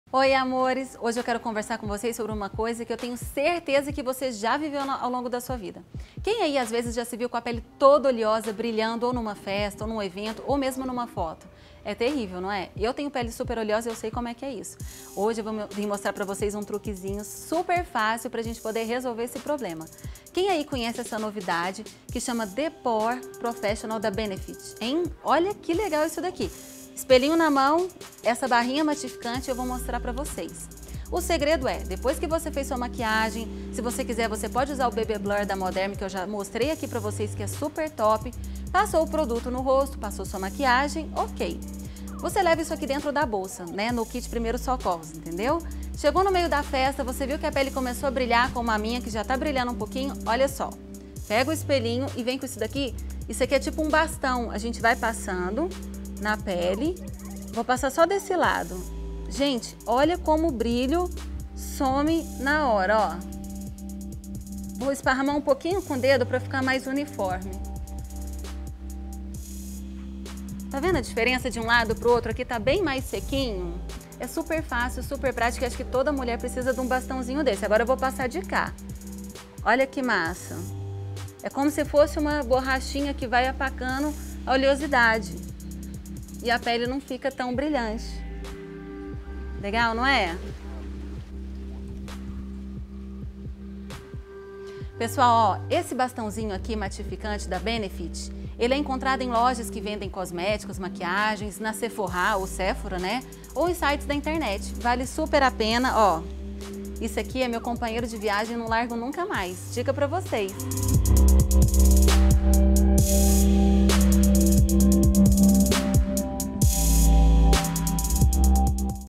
RESENHA: THE POREFESSIONAL I O MATIFICANTE EM BASTÃO DA BENEFIT
Oi, amores! Hoje vim falar pra vocês sobre o the POREfessional, o matificante em bastão da Benefit. Minha pele é bastante oleosa, então sei bem o que é ver o rosto brilhando no meio do dia. Mesmo usando sabonete, protetor solar e base específica para nosso tipo de pele, ao longo do dia o brilho pode teimar em aparecer. Nessa hora vale ter um quebra galho pra disfarçar o brilho rapidamente, não é? No vídeo falo mais sobre o assunto.
Caso você tenha alguma companheira de pele oleosa marque ela aqui nos comentários. A nação oleosa agradece! 😉
✨ Se você tem curiosidade para ler mais resenhas sobre produtos para pele oleosa, clique e